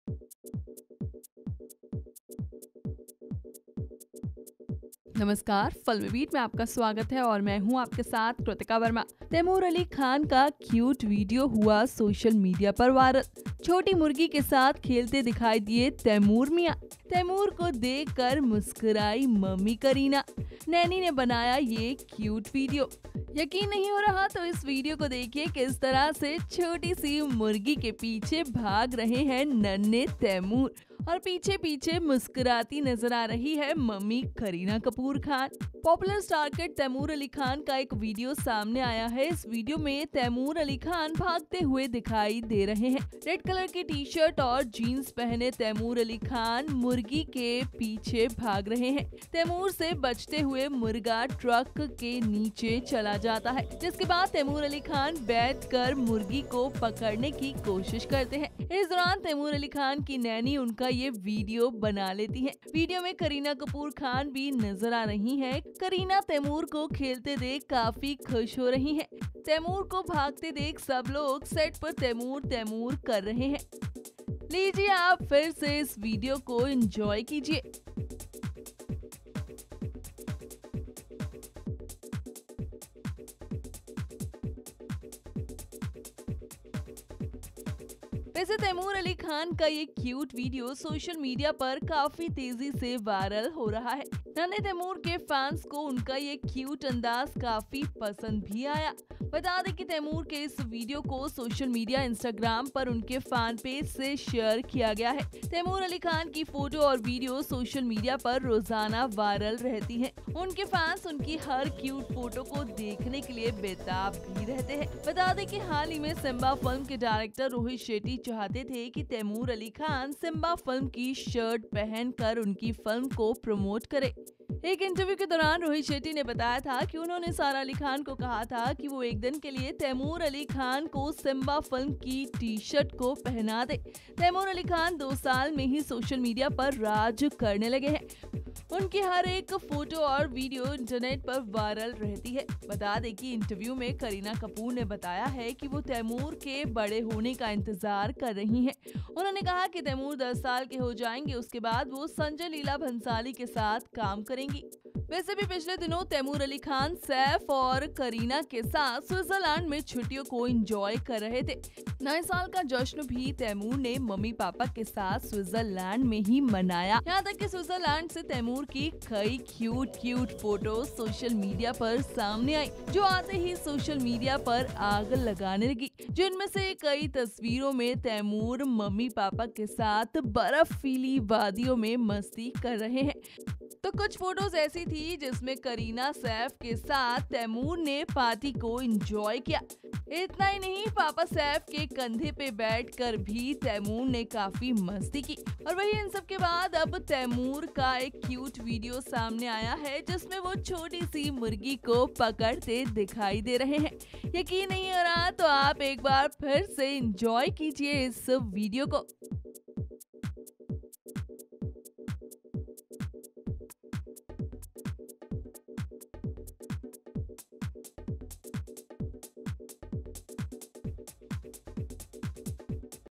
Taimur Ali Khan plays with little chicken; Kareena Kapoor Khan smiles on ; Watch video | FilmiBeat
Taimur Ali Khan enjoys with little chicken; Kareena Kapoor Khan smiles on. . In the video that is now doing the rounds of social media, we see little Taimur running after a chicken. While he's busy running, his mom, Kareena Kapoor Khan can be seen following him with a cup of tea in her hands on what looks like a set. Watch the video to know more !